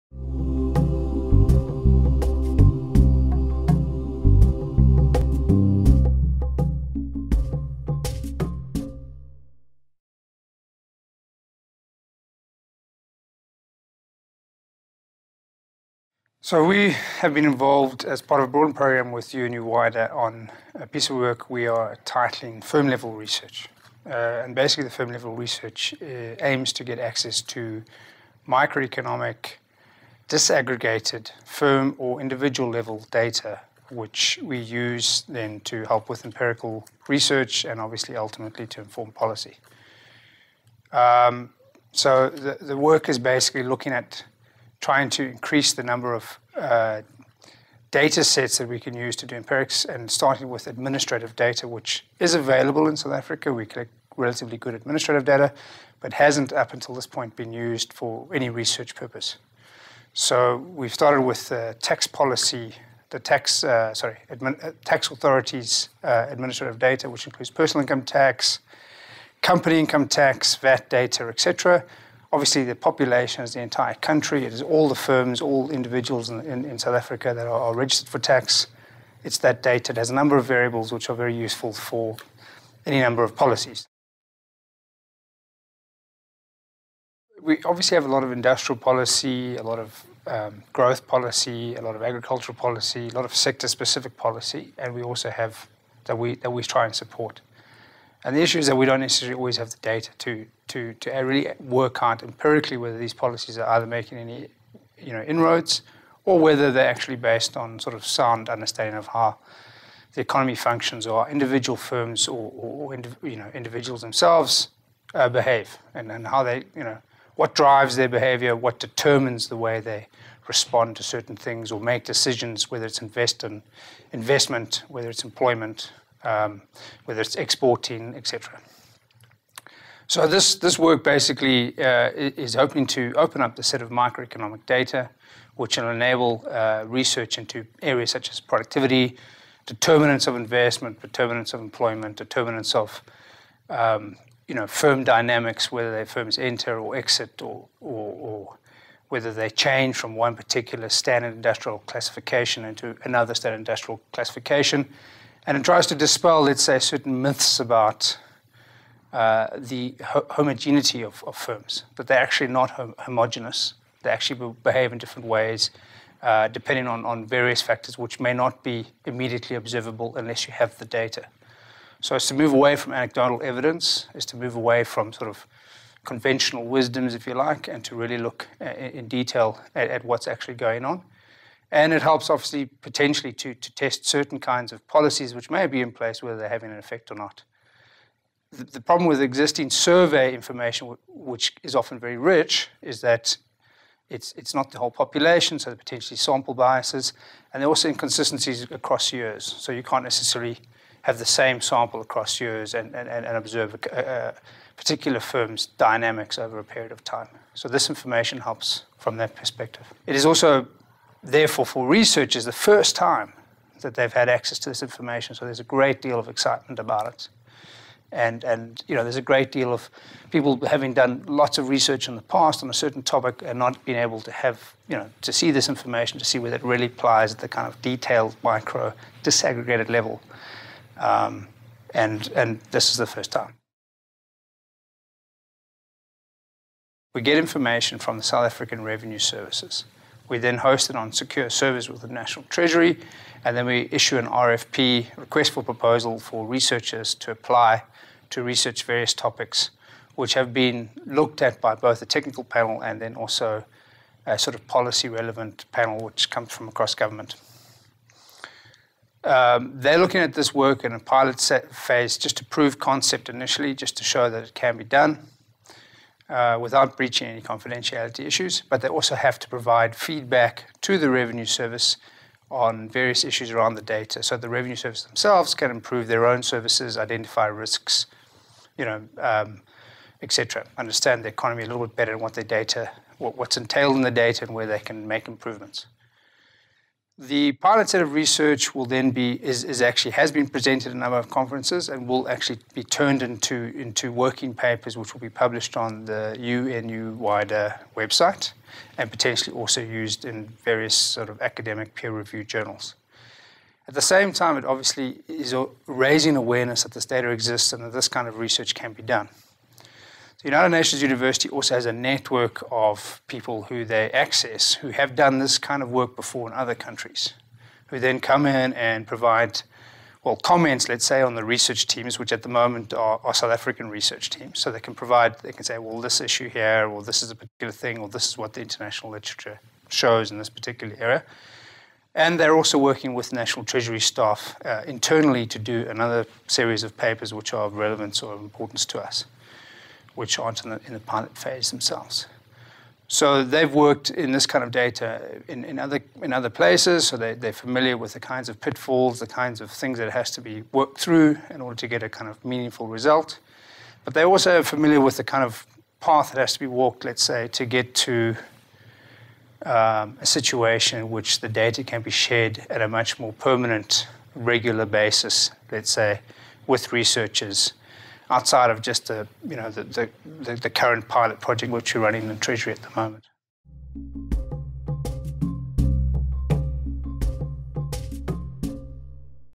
Using tax data to inform policy - An interview with Landon McMillan
In this video Landon McMillan of the National Treasury of the Republic of South Africa explains the joint work done with UNU-WIDER in the area of firm and industry level research in South Africa.

This collaborative research aims to get access to microeconomic disaggregated firm or individual level data which haven’t been used before for empirical analysis in South Africa.

0:15 What work have you been involved with UNU-WIDER?
1:26 What are you trying to achieve and why is it exciting?
3:56 How is the research organized?